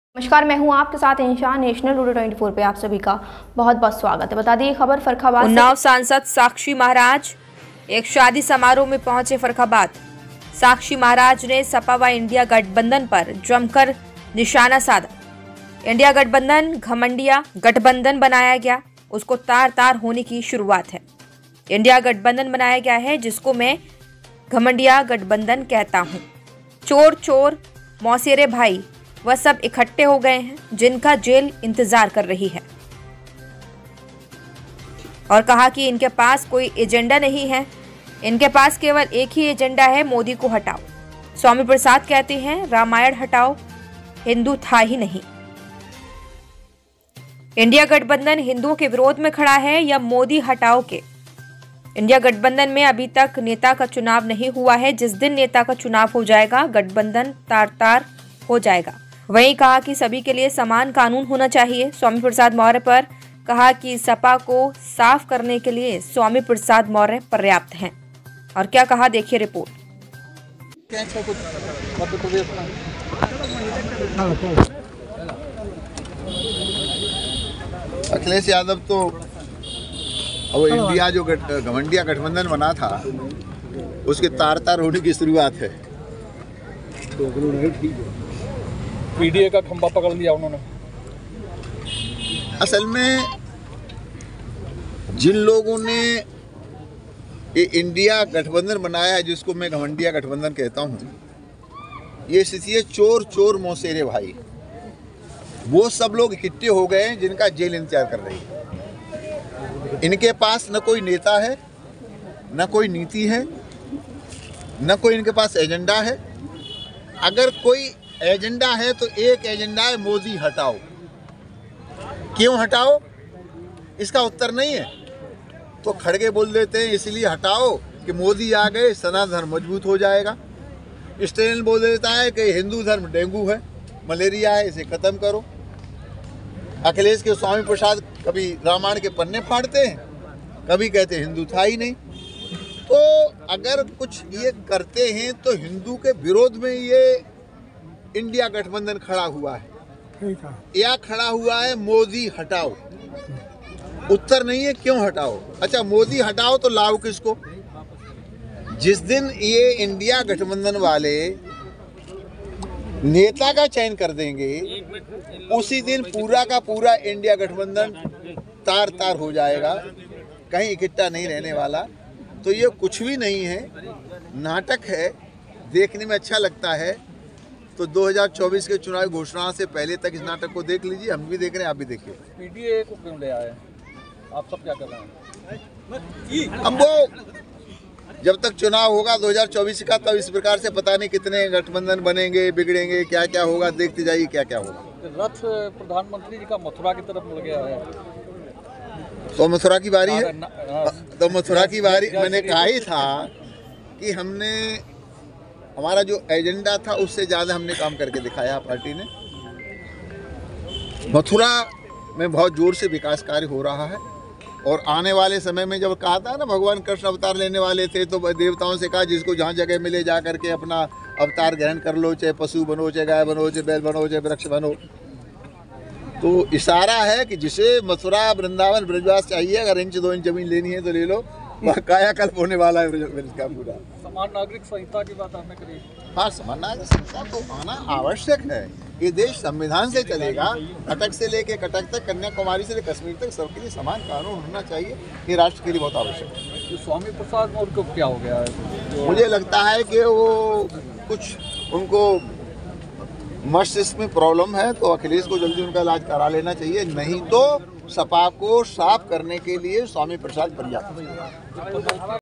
उन्नाव सांसद साक्षी महाराज एक शादी समारोह में पहुंचे, सपा व इंडिया गठबंधन पर साधा निशाना
फर्रुखाबाद में उन्नाव सांसद साक्षी महाराज एक शादी समारोह में पहुंचे, साक्षी महाराज ने सपा व इंडिया गठबंधन पर साधा निशाना।